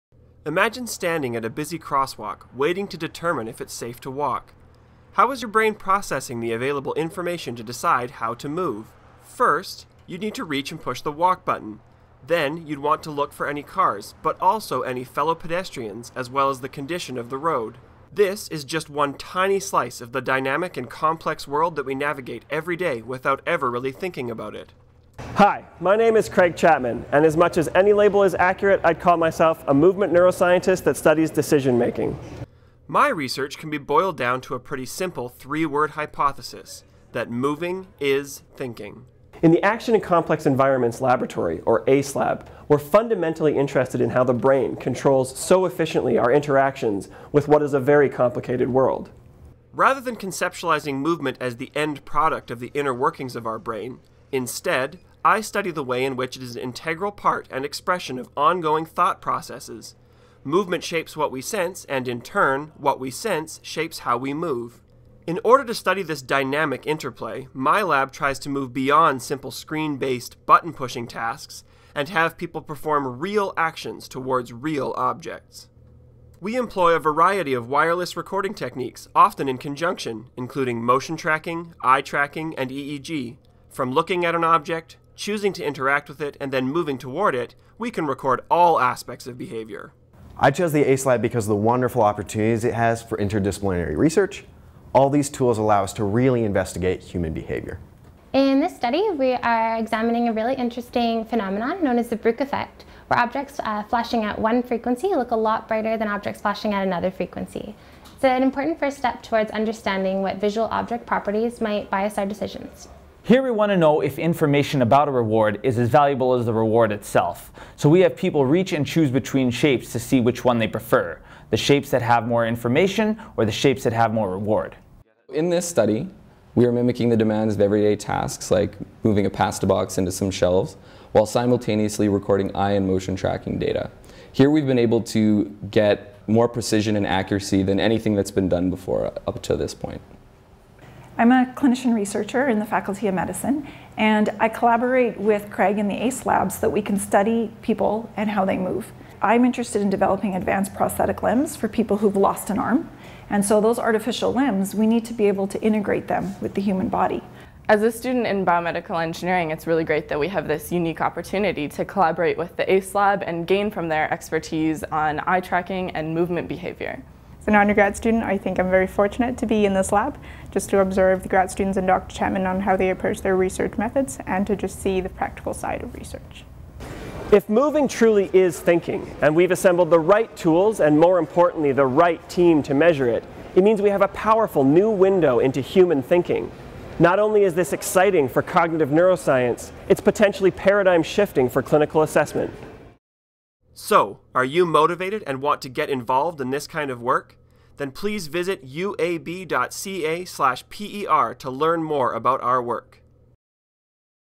Dr. Craig Chapman Assistant Professor Video
Dr. Craig Chapman and his team in the ACELab are fundamentally interested in learning about how our brain so efficiently controls our interactions. To learn more about Dr. Chapman's research, visit uab.ca/per